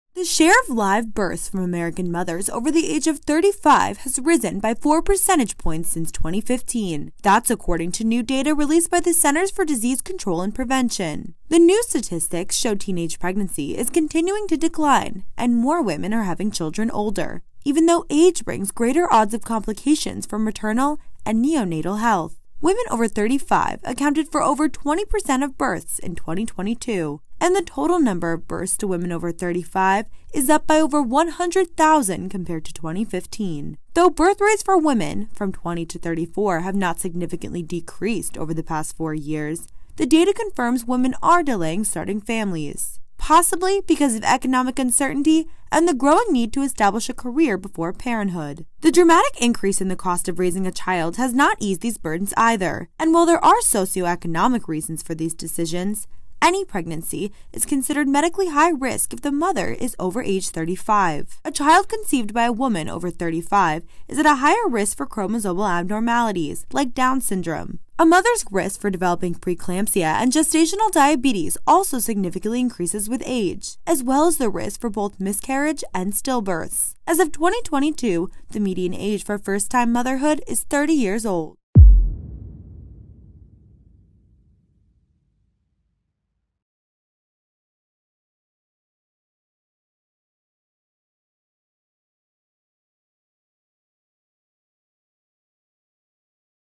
Maternal age rising as birthrate declines, new data show
The share of live births from American mothers over the age of 35 has risen by 4 percentage points since 2015, according to new data released Thursday by the Centers for Disease Control and Prevention.

The new statistics show teenage pregnancy is continuing to decline and that more women are having children older, when there are greater odds of complications for maternal and neonatal health.